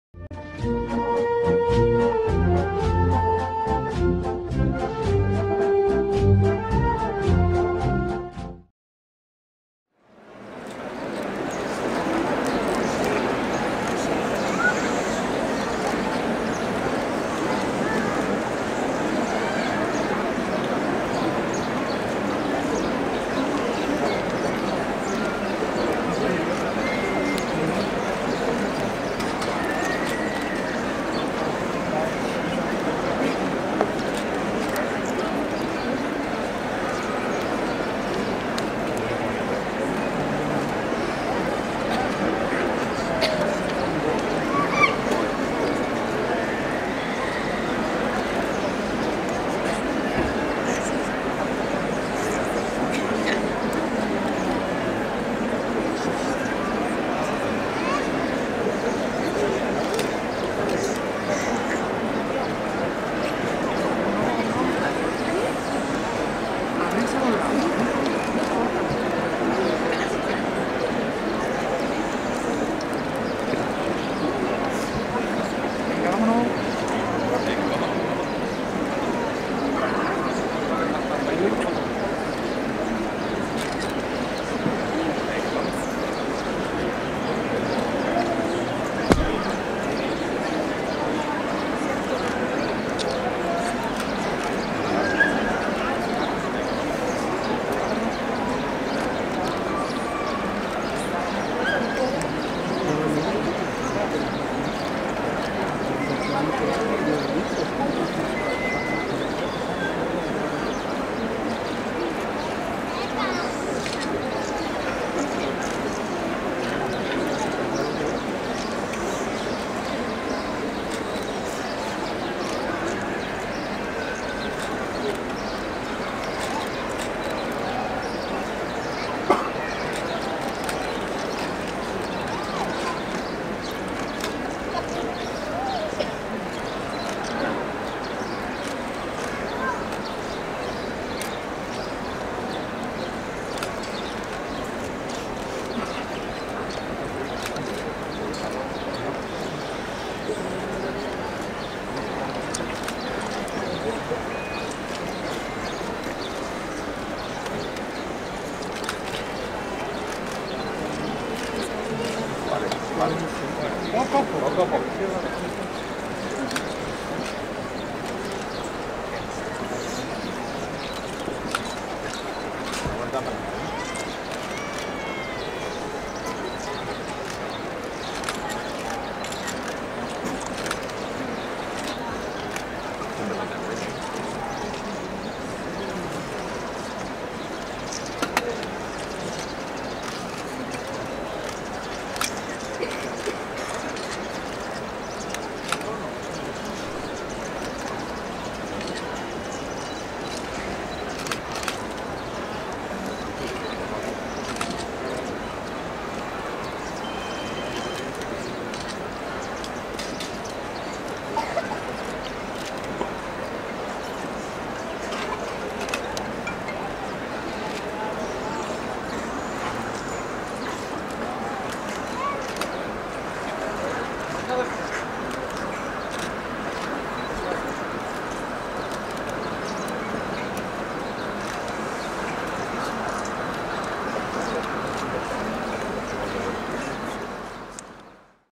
Salida del Cristo de la Buena Muerte | Los Estudiantes | Semana Santa 2018
Momentos de la salida del Cristo de la Buena Muerte de los Estudiantes, desde el rectorado de la universidad, la tarde del martes santo de 2018.

Graba: Miguel Angel López Galán